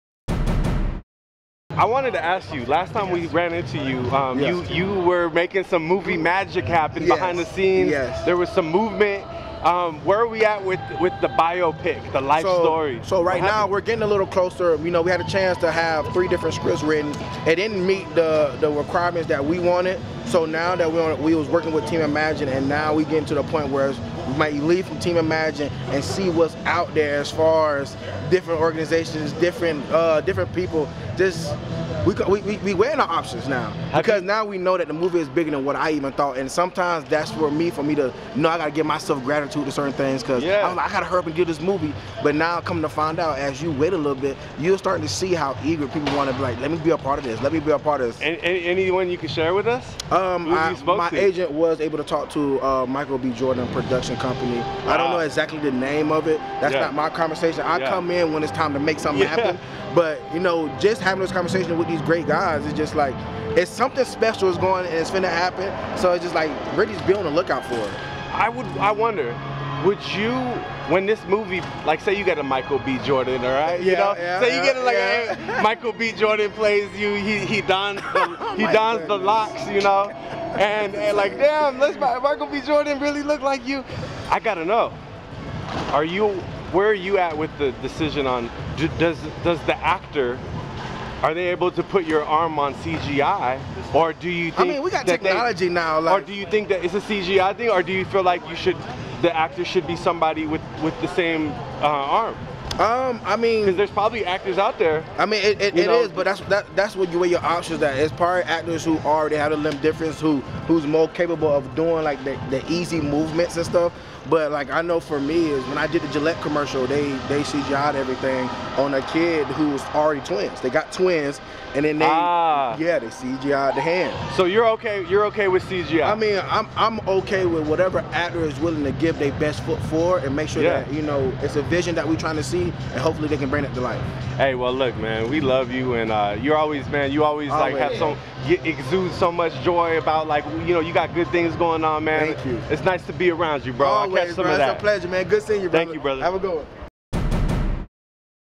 Shaquem Griffin Shopping Biopic, In Early Talks With Michael B. Jordan | TMZ Sports
Shaquem Griffin says interest in a biopic about his life has recently exploded ... so much so, he tells TMZ Sports Michael B. Jordan has shown interest in acquiring it!!